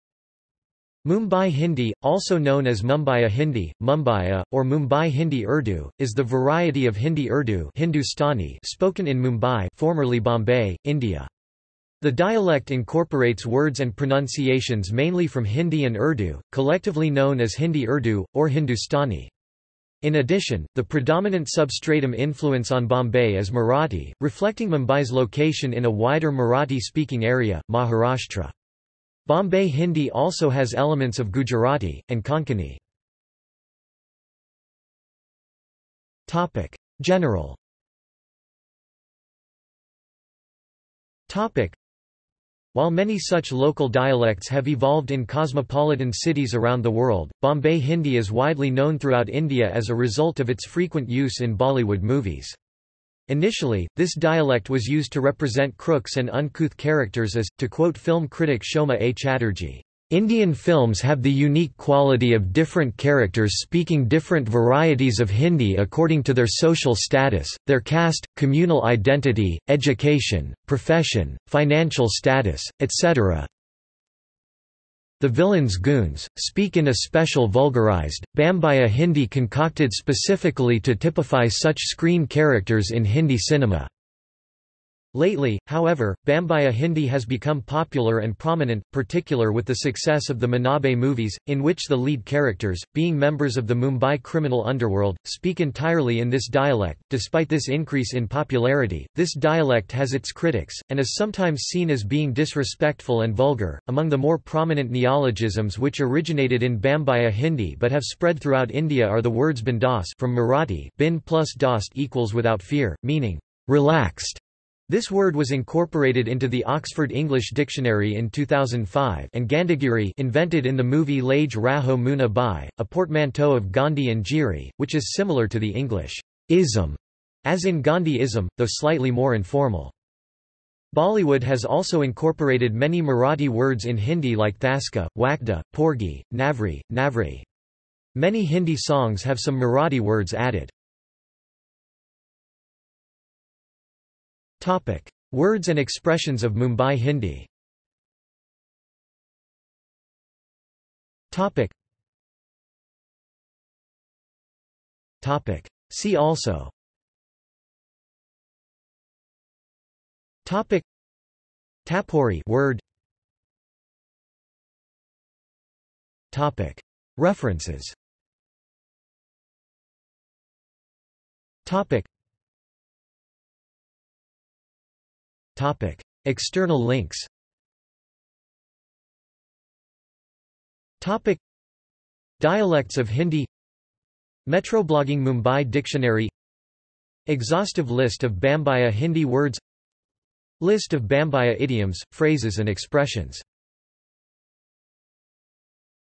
Bombay Hindi | Wikipedia audio article
This is an audio version of the Wikipedia Article:
Bombay Hindi

SUMMARY
Mumbai Hindi, also known as Mumbaiya Hindi,  Mumbaiyya, or Mumbai Hindi-Urdu, is the variety of Hindi-Urdu (Hindustani) spoken in Mumbai (formerly Bombay), India. The dialect incorporates words and pronunciations mainly from Hindi and Urdu, collectively known as Hindi-Urdu, or Hindustani. In addition, the predominant substratum influence on Bombay is Marathi, reflecting Mumbai's location in a wider Marathi-speaking area, Maharashtra. Bombay Hindi also has elements of Gujarati, and Konkani.